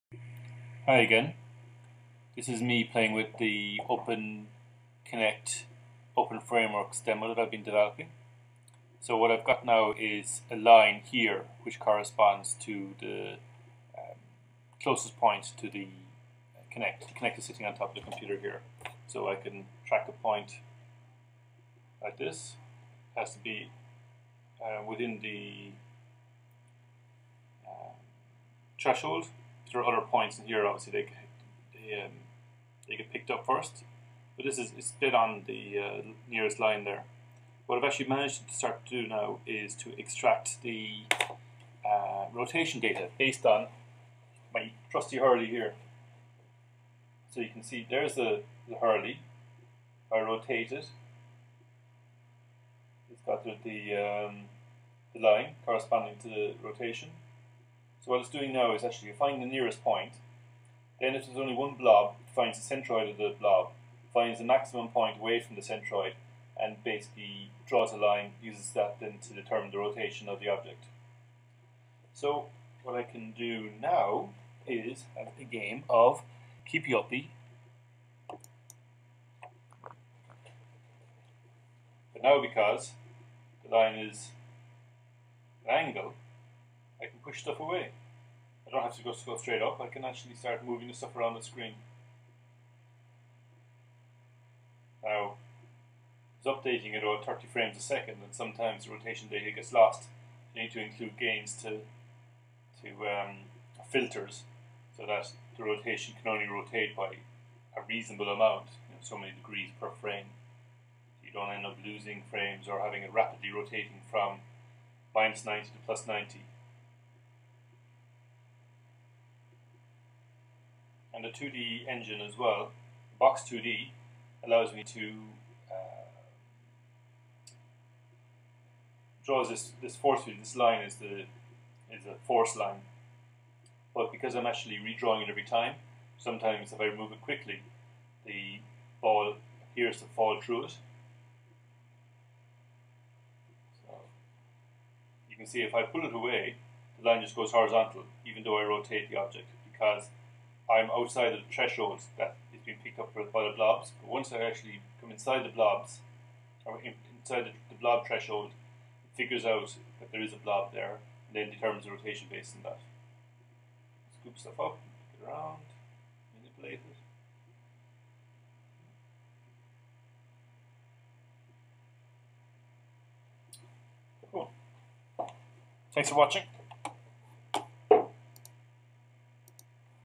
Virtual hurling with the xbox kinect
This shows an example of how the rotation of objects seen by the kinect can be extracted and used to modify virtual objects within the 2d world .